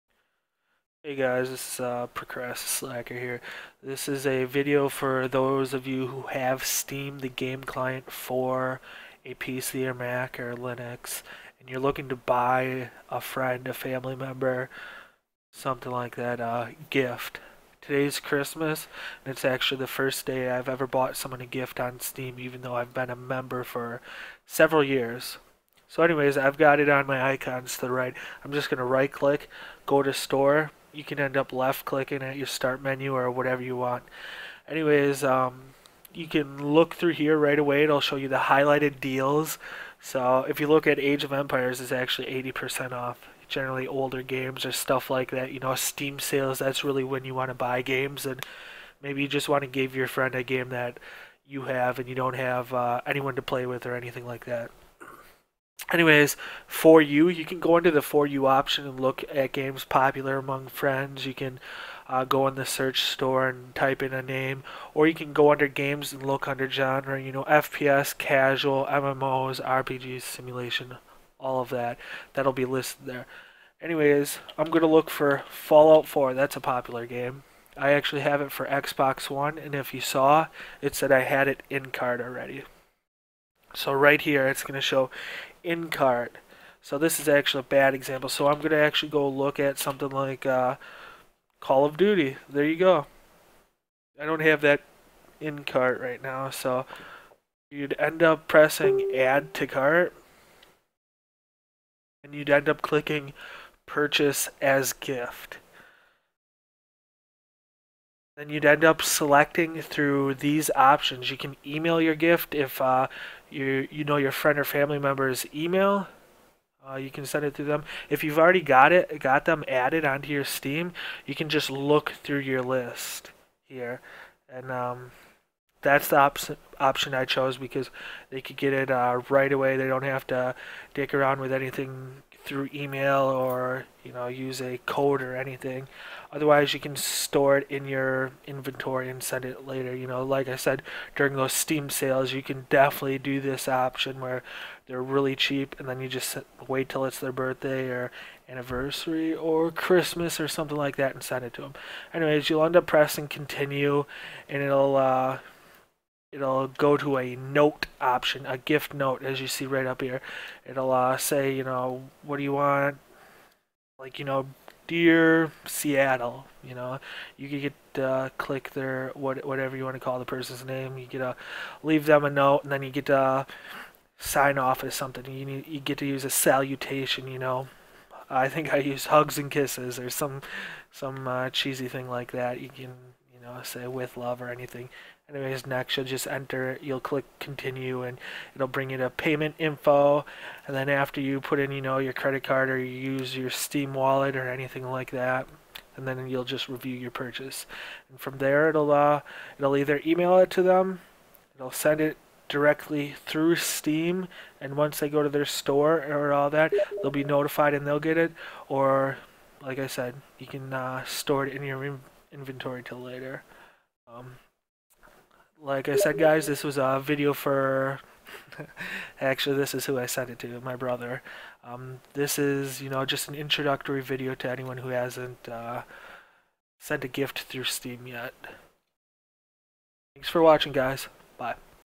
How to buy a game & gift it to someone on Steam
This is an entry level tutorial on how to buy a game in Steam & gift it to a friend!